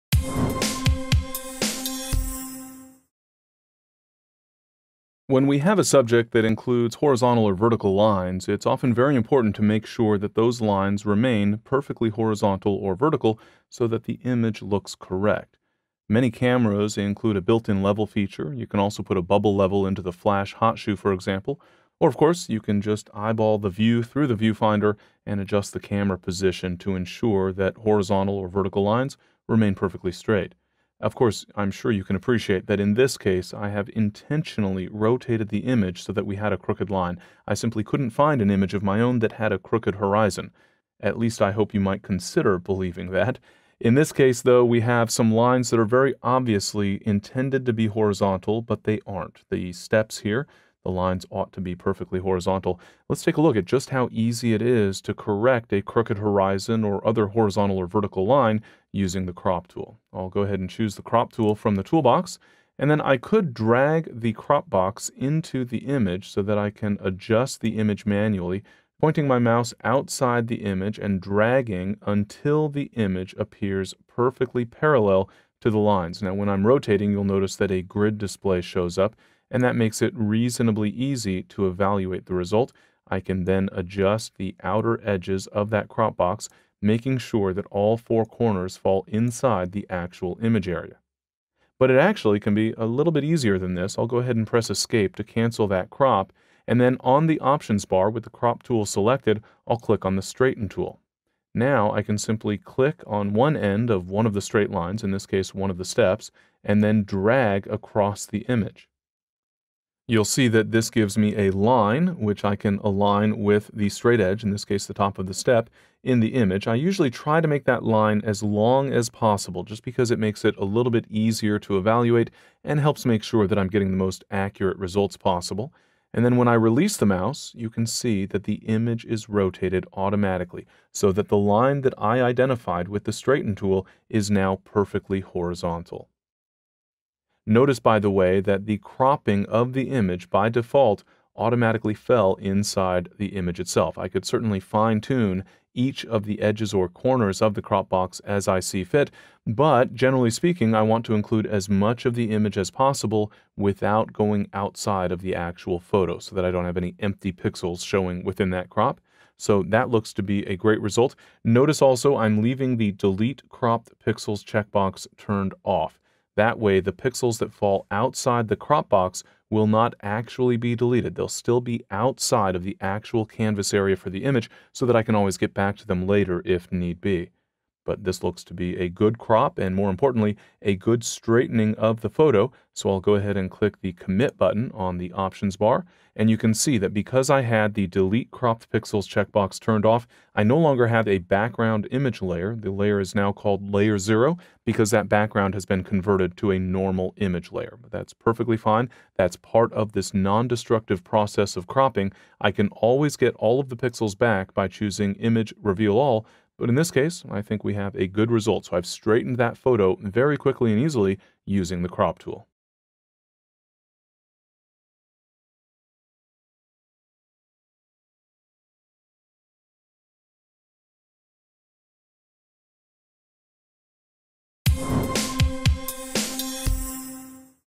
Straightening a Crooked Image
In this free lesson from video2brain's course, Photoshop CS6 Image Cleanup Workshop, Tim Grey demonstrates how to quickly and easily straighten a crooked photo, because when a photo includes lines that should be perfectly horizontal or vertical, it is important that those lines be true. For more information on this course, please